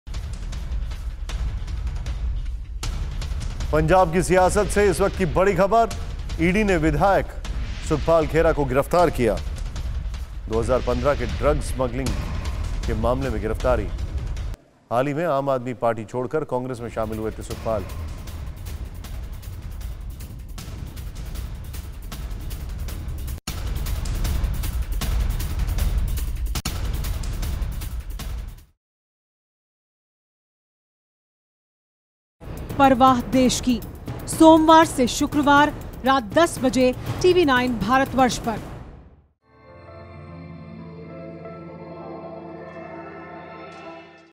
AAP छोड़कर कांग्रेस में शामिल हुए पूर्व विधायक Sukhpal Khaira गिरफ्तार
आम आदमी पार्टी छोड़कर हाल ही में कांग्रेस में शामिल हुए पूर्व विधायक Sukhpal Khaira  को साल 2015 के ड्रग स्मगलिंग के एक मामले को लेकर ED ने जालंधर में गिरफ्तार किया. इसी मामले को लेकर जारी पूछताछ के दौरान सुखपाल खैहरा को गिरफ्तार किया गया है.